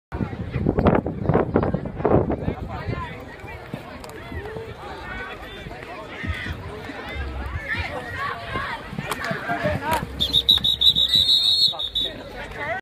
West Mesquite Spring Football Game
West Mesquite senior Curtis Williams picks up some tough yardage during the Wranglers' spring game.